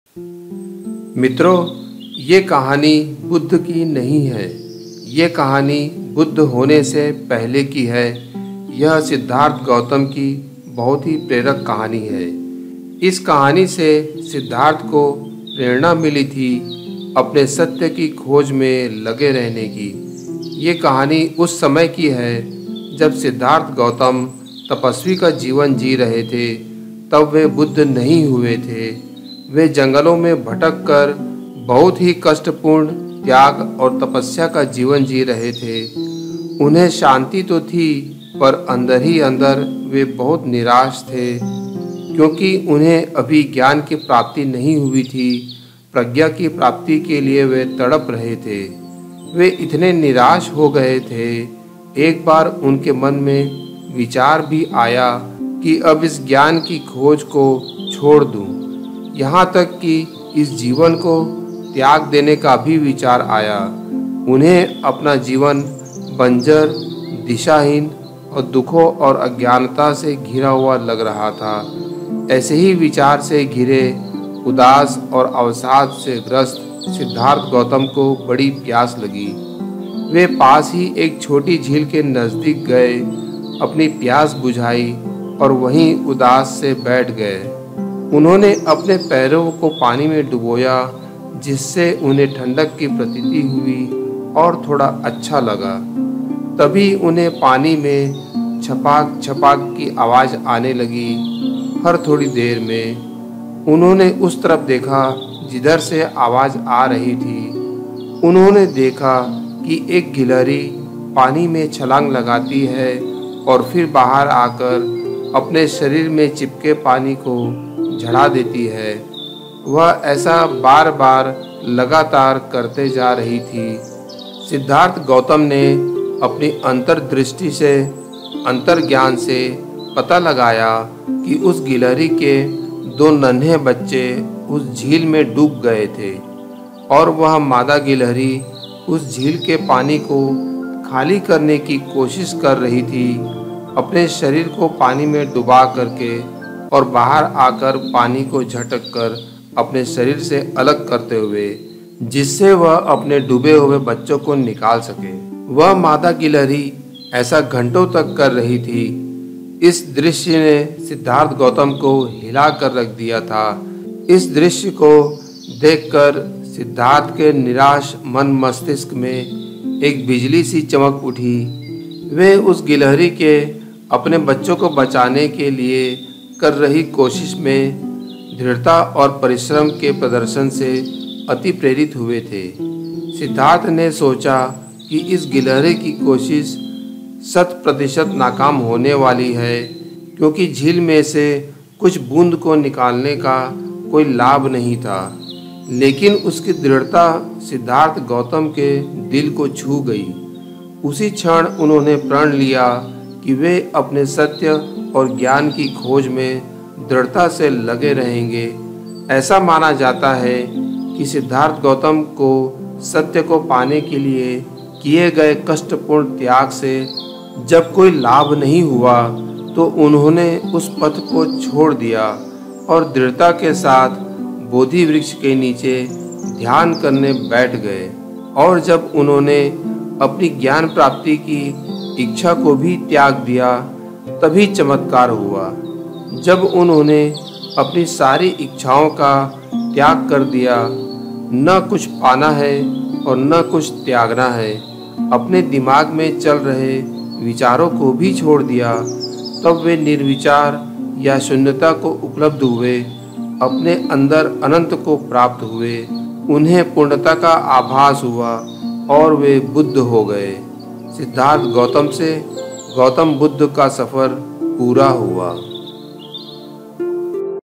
सिद्धार्थ गौतम से गौतम बुद्ध का सफ़र | BUDDHA STORY
जब सिद्धार्थ को जीवन त्यागने का विचार आया , तब किससे उन्हें प्रेरणा मिली .